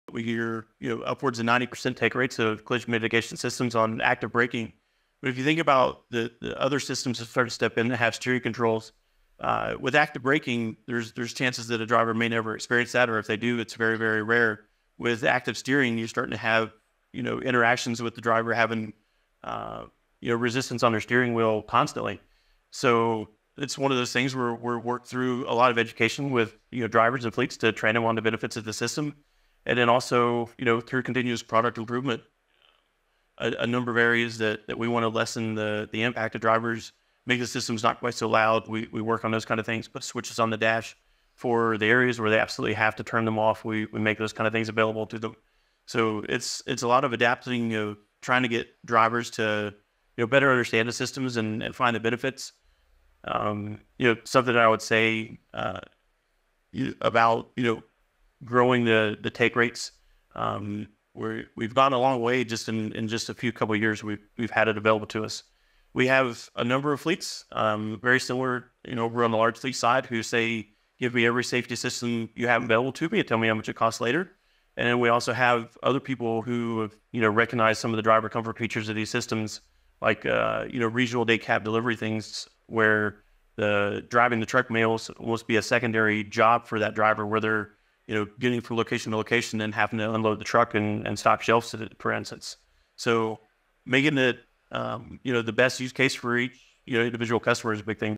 DTNA’s Michael Young: Educate drivers, improve systems, and tailor solutions to boost tech adoption
GUESTS
▪ Johan Agebrand, Director of Product Marketing, Volvo Trucks North America
▪ Sunny Choi, VP of Global Business Development, Plus
▪ Kevin Chronicle, Sales Director - Commercial Vehicles, Bosch
▪ Michael Young, Product Marketing Manager, Daimler Truck North America

MODERATOR
▪ Wiley Deck, VP of Government Affairs and Public Policy, Plus

Safety is a top priority for us across the industry and a topic we at Plus discuss all the time. In this panel discussion, we delved into the world of ADAS technology, discussing the industry’s shared vision for enhancing safety, the tech advances available today for fleets, and the impact of NHTSA’s AEB mandate. We also touched on what next-generation ADAS will look like, the transition to higher levels of autonomy, and the steps fleets can take to get ahead of the curve.